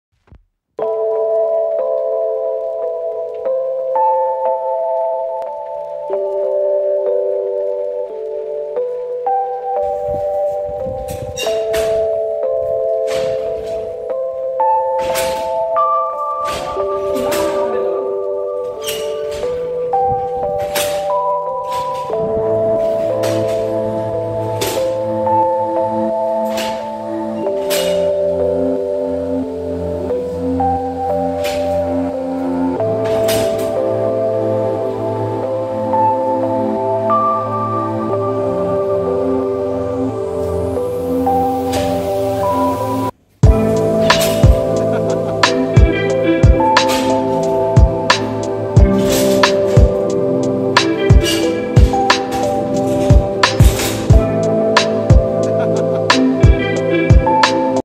point set lock&signal change cabinman यार्ड में केबिनमैन द्वारा प्वाइंट सेट और लॉक सिग्नल परिवर्तन
रेल यार्ड में केबिनमैन द्वारा प्वाइंट सेट और लॉक सिग्नल परिवर्तन

Real Hard work of the cabinman under Traffic department . lever Pulling scenes to make a point set and lock with signal interlocking system

Alternative of RRI, old primitive system of point settings